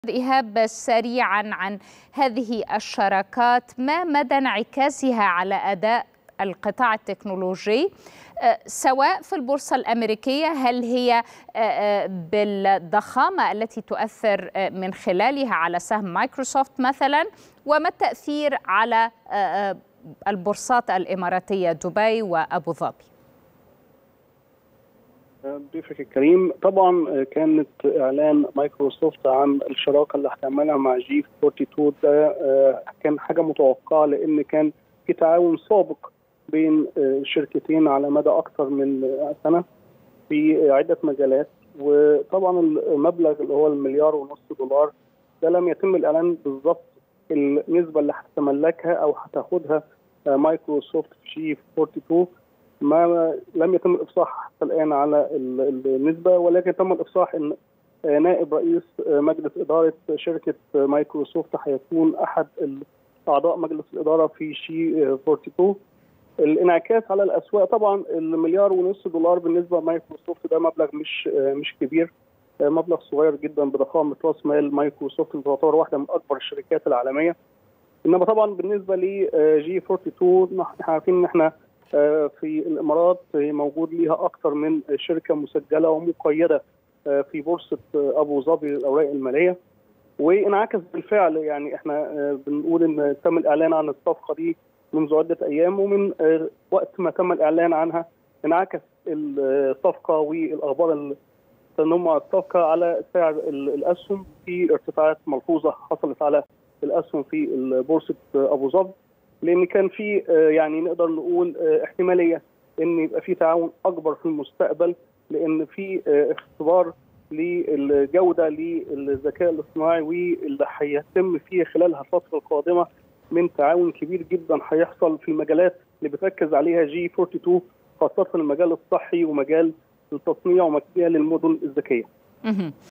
كيف ستؤثر صفقة شراكة مايكروسوفت وG42 على أداء القطاع التكنولوجي في البورصة؟
كيف ستؤثر صفقة شراكة مايكروسوفت وG42 على أداء القطاع التكنولوجي في البورصة؟.. إيهاب رشاد محلل أسواق المال الخليجية يُجيب

22/4/2024